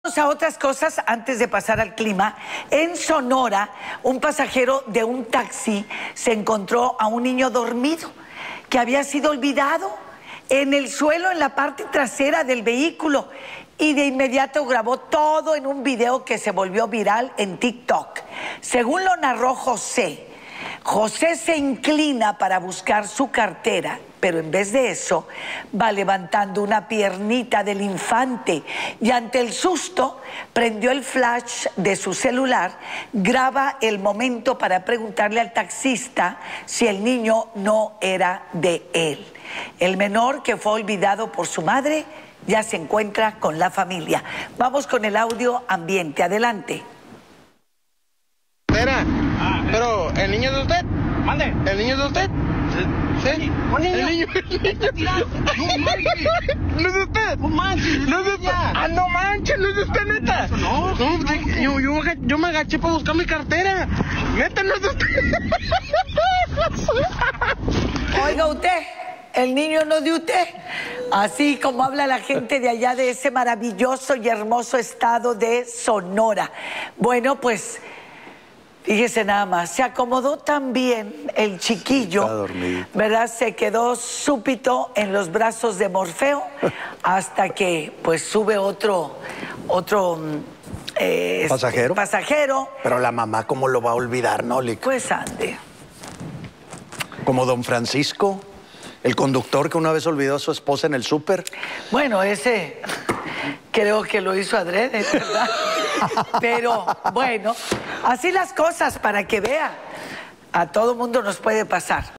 Olvidan a niño en taxi en Sonora
José compartió un video en el que al momento de buscar su cartera, luego de que se le cayera en el taxi en el que viajaba encontrara a un niño durmiendo.

Programación
Lunes a viernes:
Telediario Matutino de 05:00- 10:00
Telediario Mediodía de 12:00-14:30
Telediario Nocturno de 19:00- 21:00